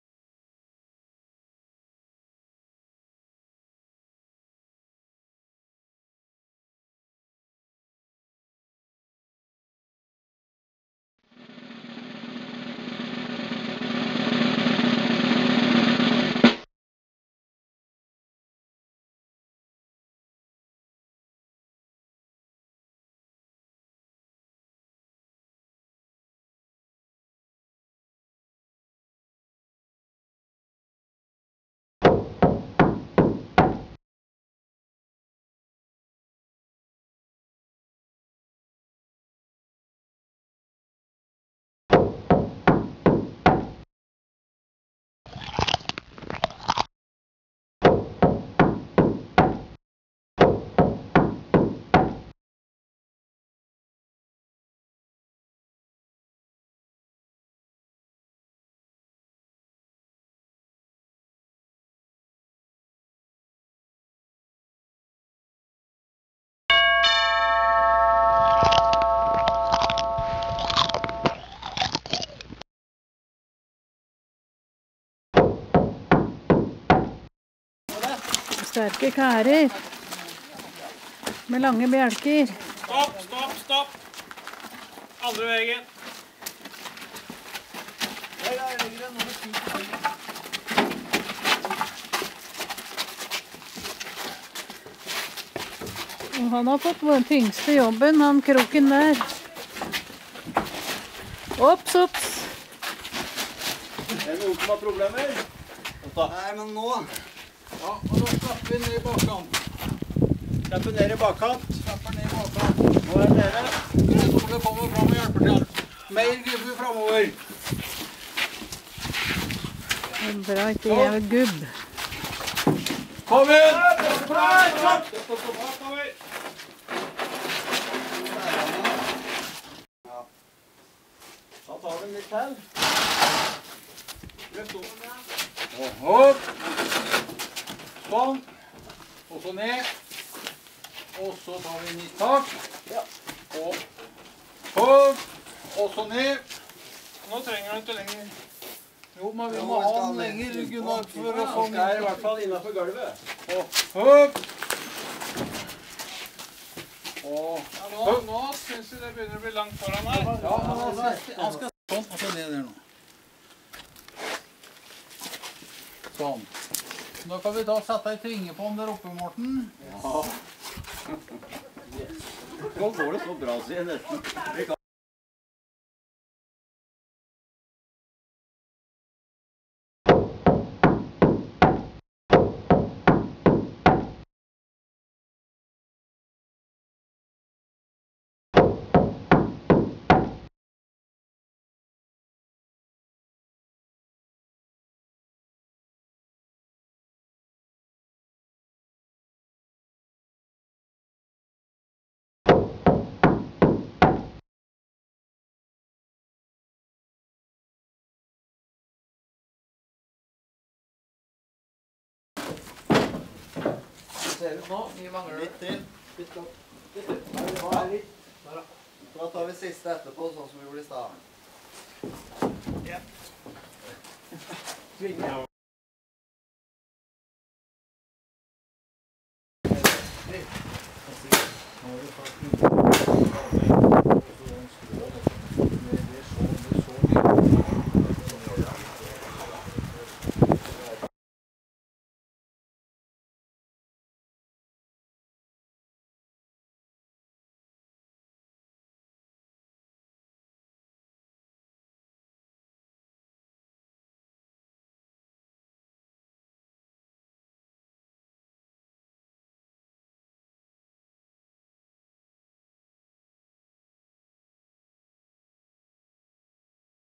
Hyttebygging Trysil
Elementene i en Saltdals hytte settes opp på 2 dager. Osin ligger på østsiden av Osen sjøen i Trysil. Bilder i serie er tatt med 1 min. mellomrom.